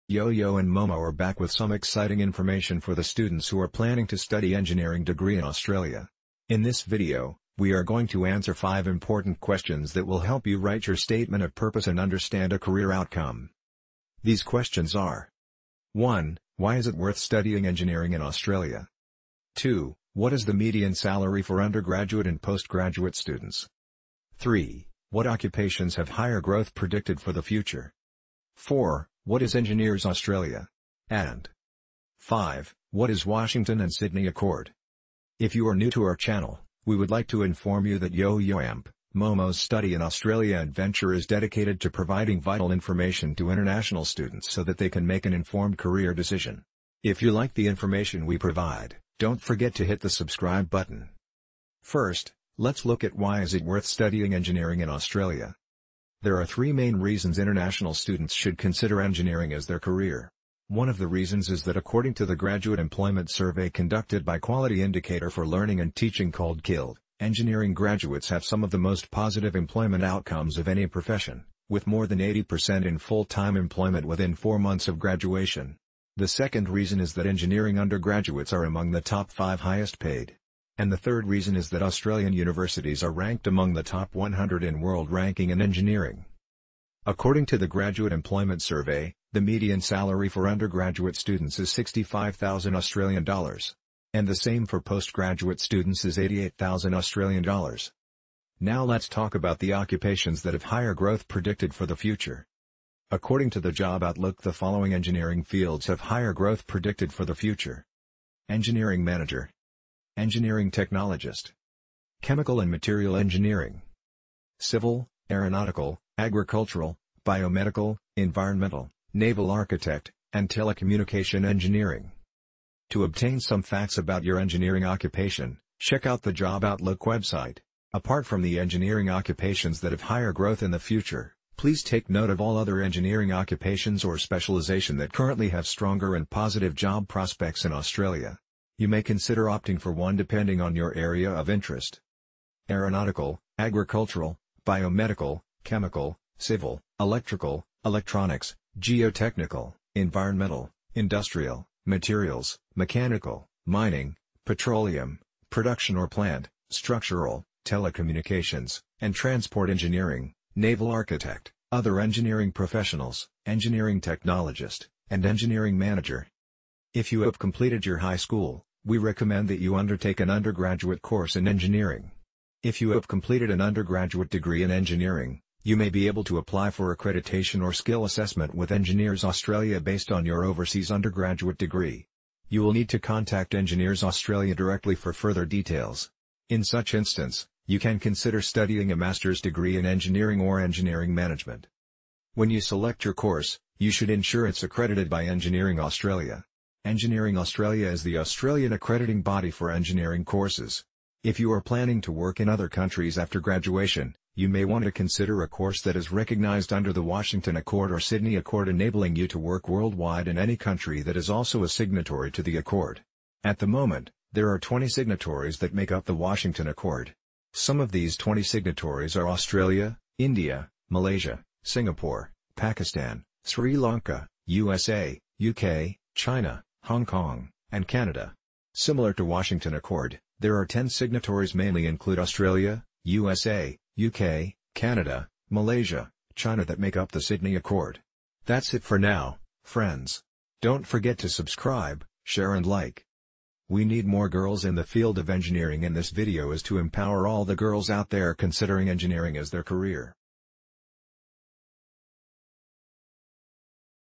How to become an Engineer? | Engineering Courses | Engineering Occupations | Study in Australia
In this video, we are going to answer five important questions that will help you write your Statement of Purpose and understand a career outcome.
These questions are,
Why is it worth studying Engineering in Australia?
What is the median salary for undergraduate and postgraduate students?
What occupations have higher growth predicted for the future?
What is Engineers Australia?
What is Washington and Sydney accord?

If you are choosing to study in Australia, Engineering is a great option in order to secure a high paying job. Some Engineering courses are recognised world wide if they are part of Washington Accord or Sydney Accord.

Watch this video if you want to find out how to become an engineer in Australia.